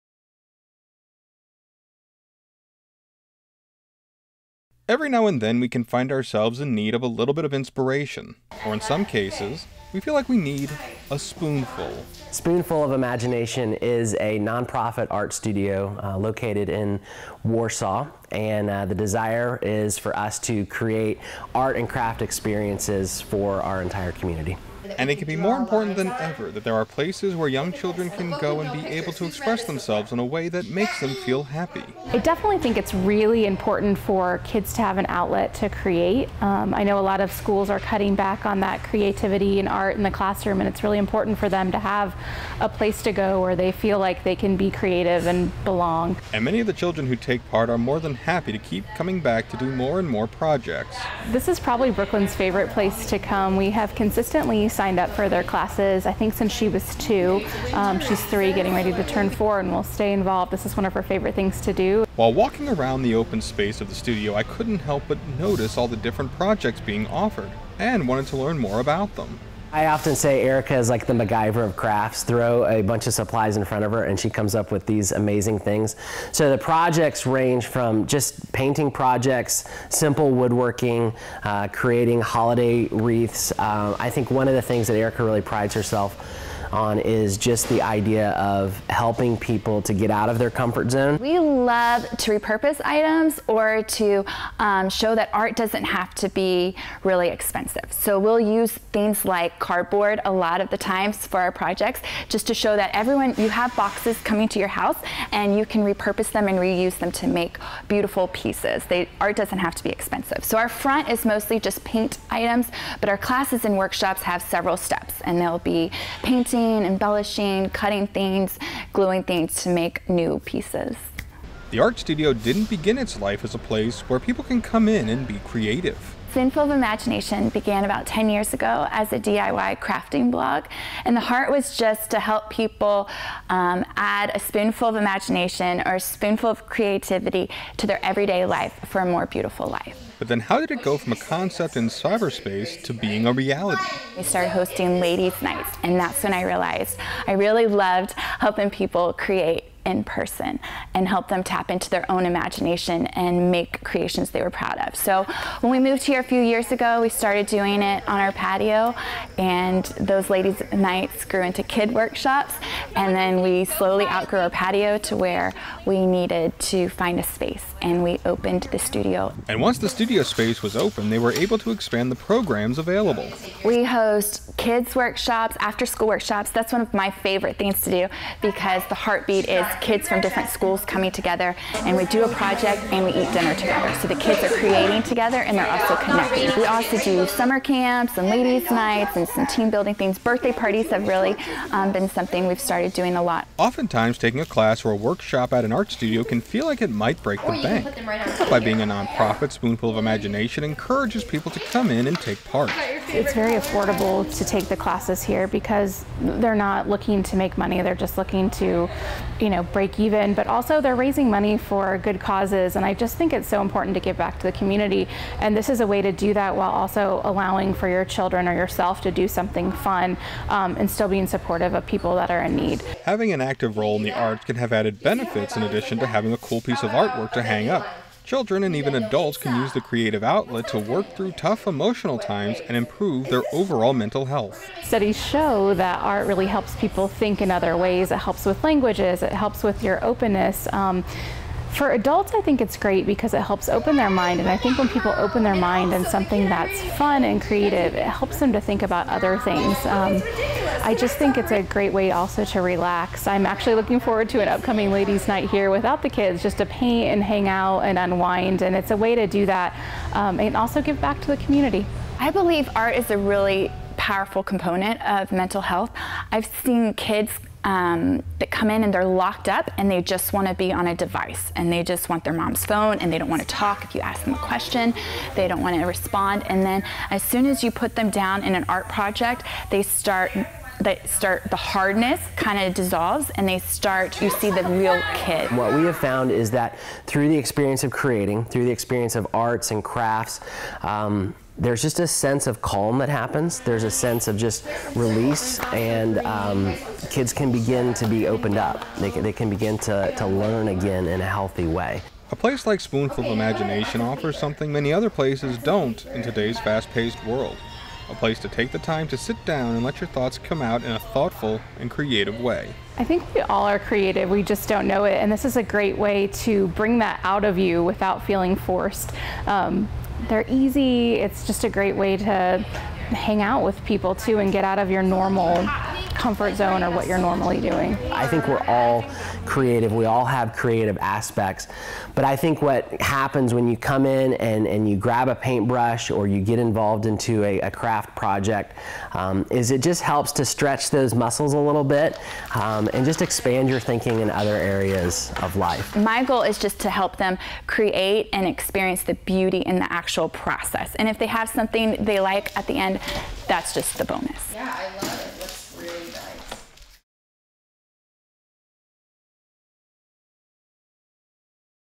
Education Counts Michiana - Spoonful of Imagination
Spoonful of Imagination is a non-profit art studio located in Warsaw, Indiana. Kids and adults have the opportunity to come in and take classes, workshops, or just have fun with arts and crafts.